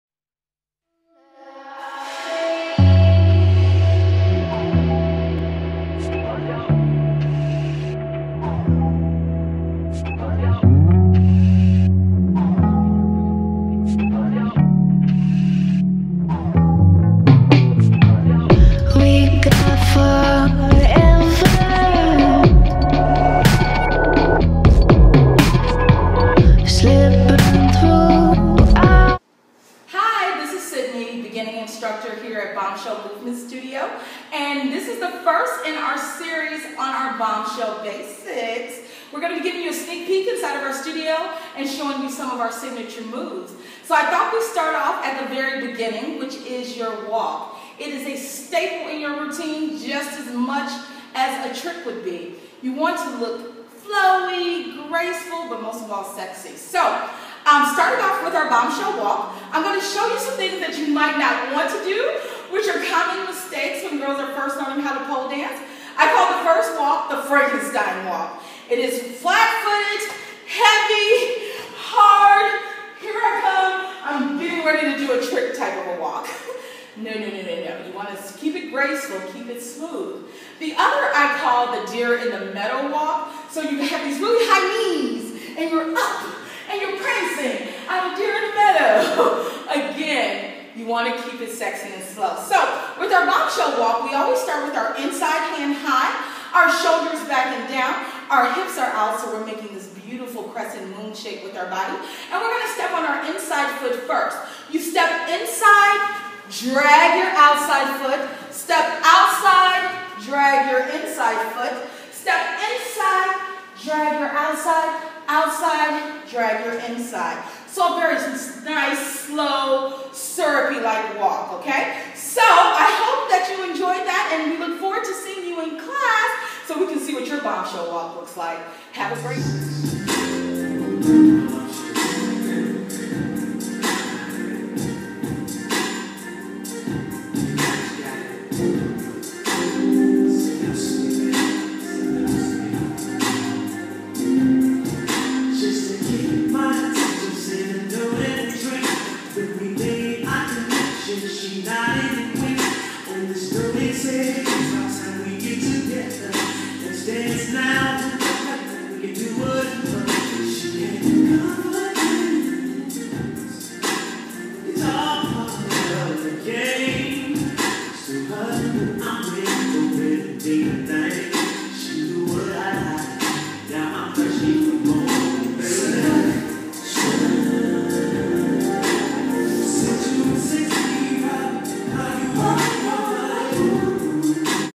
Bombshell Beginner Pole Dance Classes and Lessons
Learn to pole dance at Bombshell Movement Studio in Chicago. 10511 S Western